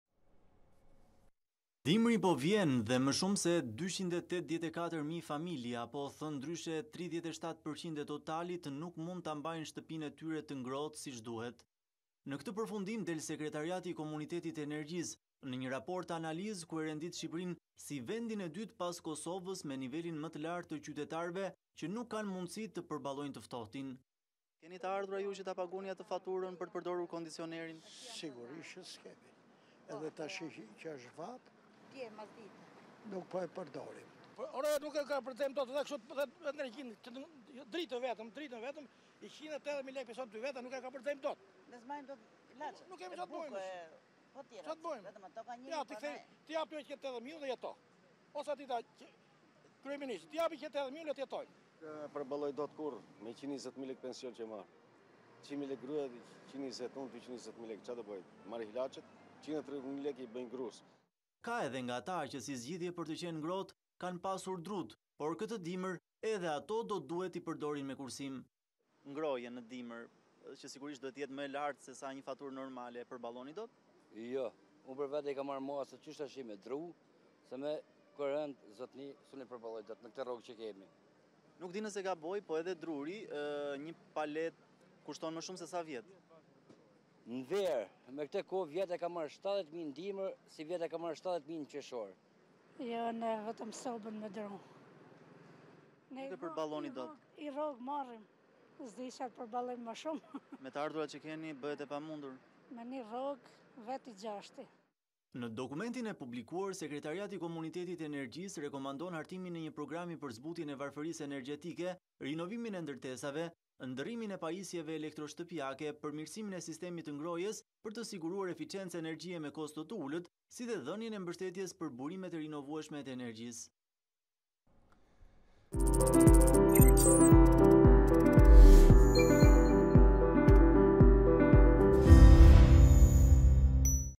Dimër i gjatë dhe i ashpër, rreth 37% e familjeve shqiptare s'mund ta mbajnë shtëpinë të ngrohtë
Dimri po vjen dhe më shumë se 284 mijë familje, apo thënë ndryshe 37 për qind e totalit nuk mund të mbajnë shtëpitë e tyre të ngrohta siç duhet. Në këtë përfundim del Sekretariati i Komunitetit të Energjisë në një raport-analizë ku e rendit Shqipërinë si vendin e dytë pas Kosovës me nivelin më të lartë të qytetarëve që nuk kanë mundësi të përballojnë të ftohtin.